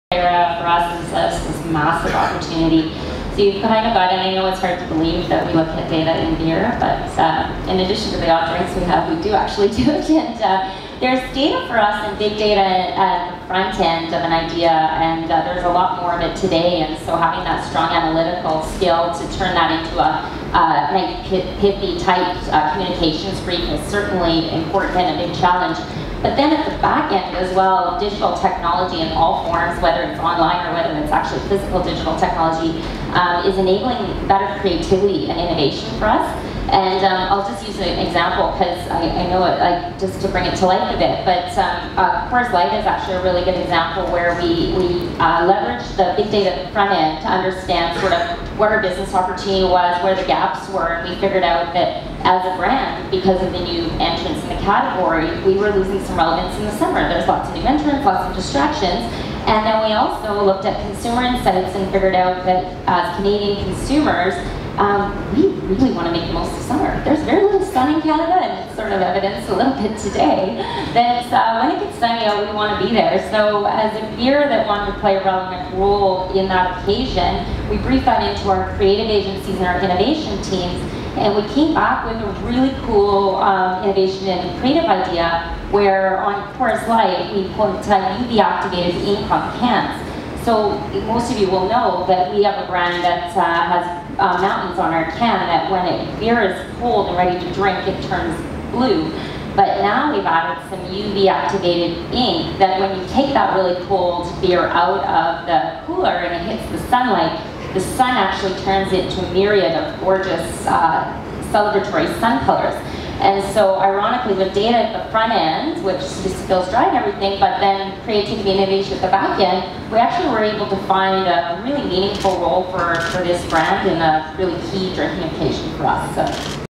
Data is enabling better creativity and innovation. Christine Jakovcic
Panel – Creative leadership in a dynamic marketing landscape

Philip Thomas CEO, Ascential Events (host)

Christine Jakovcic Chief Marketing and Consumer Excellence Officer,
MolsonCoors Canada

Howard Pyle SVP of Customer Experience and Design, MetLife

Tom Herbst Head of Global Marketing, The North Face

Richard Brown President, H&R Block Canada